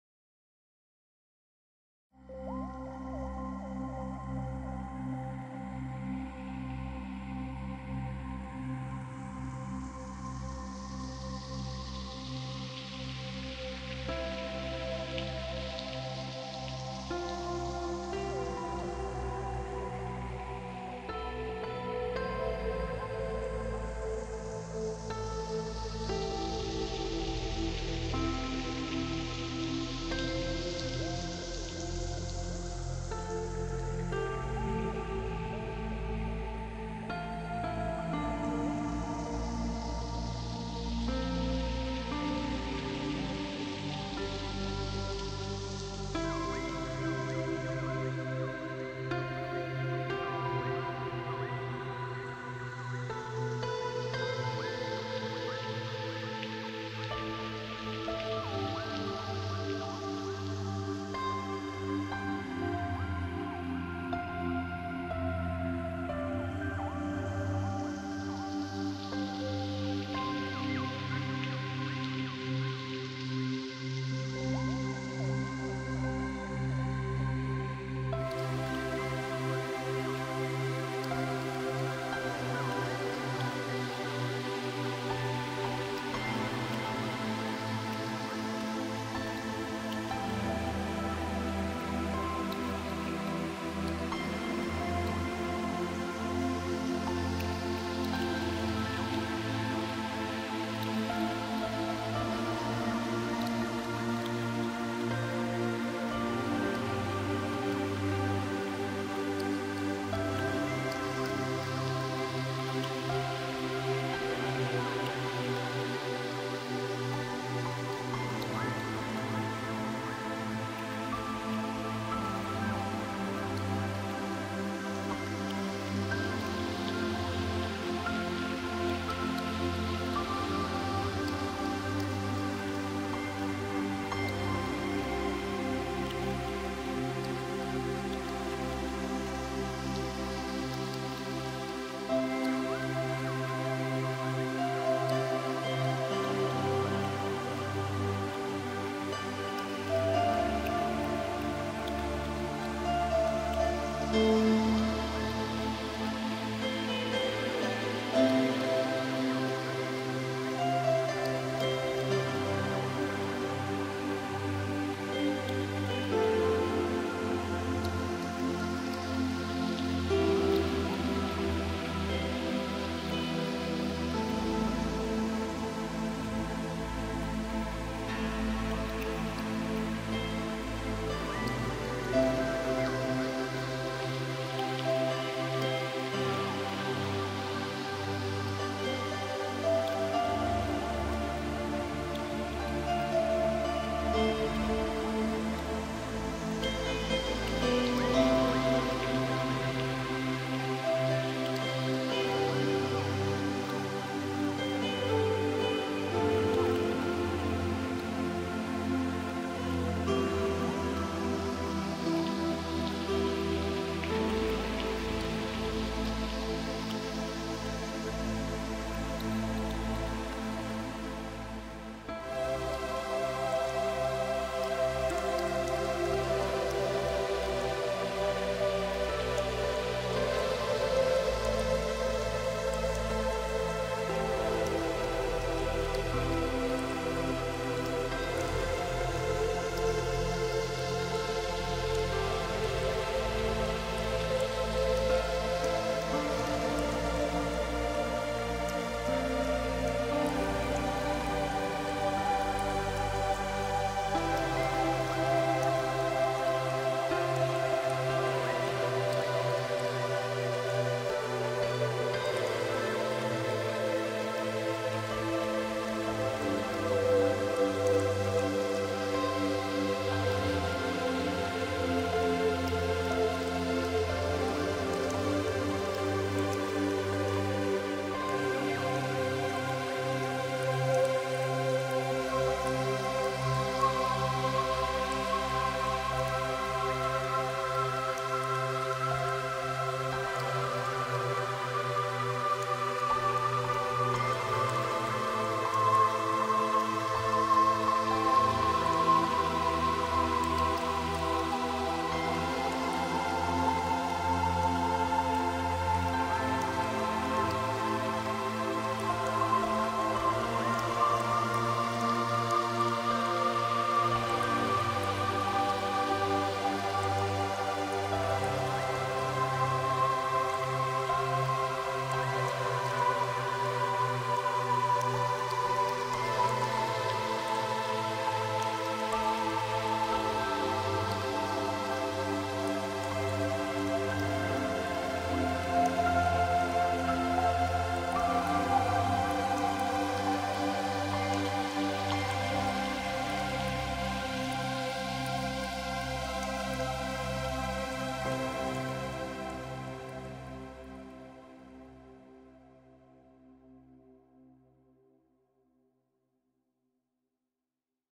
2ème évasion musicale dans le Morvan
Quelques photos de Karine Emotion fotos prises dans différents endroits du Morvan accompagnées d'une musique composée par Christophe Gambart